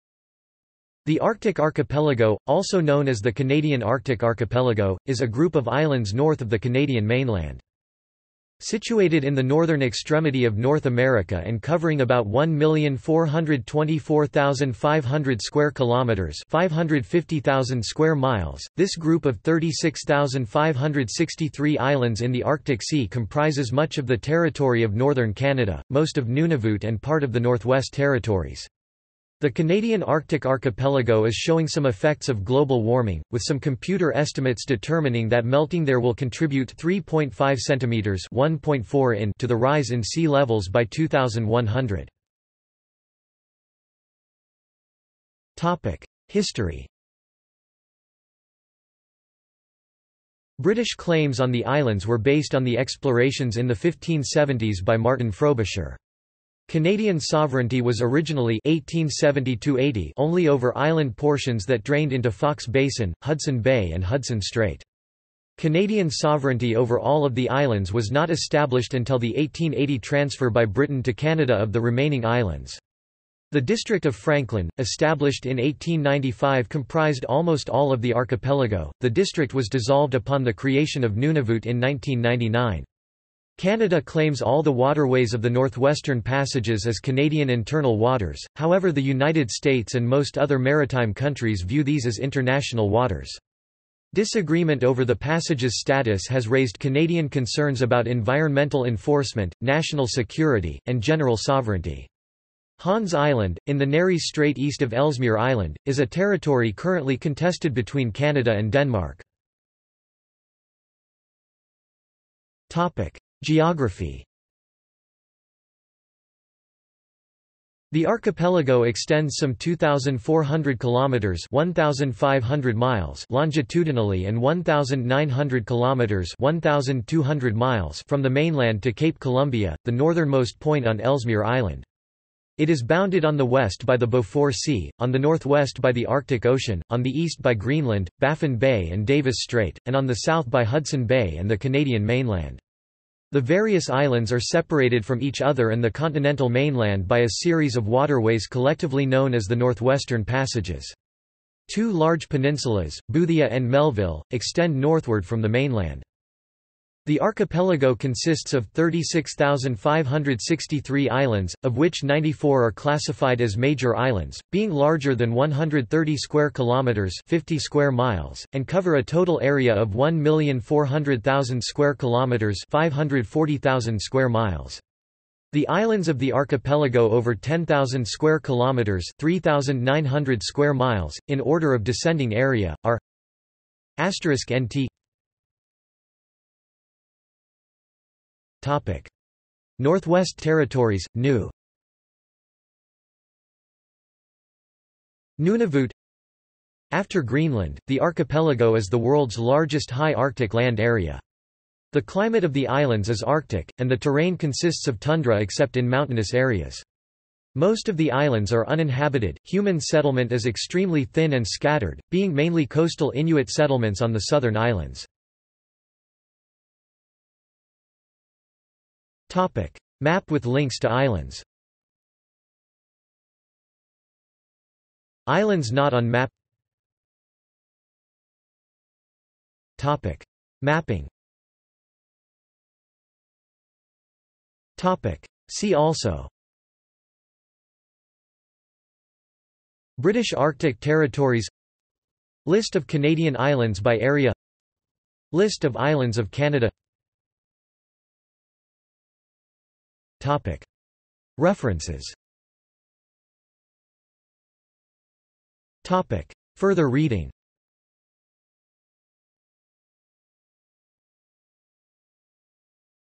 Canadian Arctic Archipelago | Wikipedia audio article
This is an audio version of the Wikipedia Article:
Canadian Arctic Archipelago

- Socrates

SUMMARY
The Arctic Archipelago, also known as the Canadian Arctic Archipelago, is a group of islands north of the Canadian mainland.
Situated in the northern extremity of North America and covering about 1,424,500 km2 (550,000 sq mi), this group of 36,563 islands in the Arctic Sea comprises much of the territory of Northern Canada – most of Nunavut and part of the Northwest Territories.  The Canadian Arctic Archipelago is showing some effects of global warming, with some computer estimates determining that melting there will contribute 3.5 cm (1.4 in) to the rise in sea levels by 2100.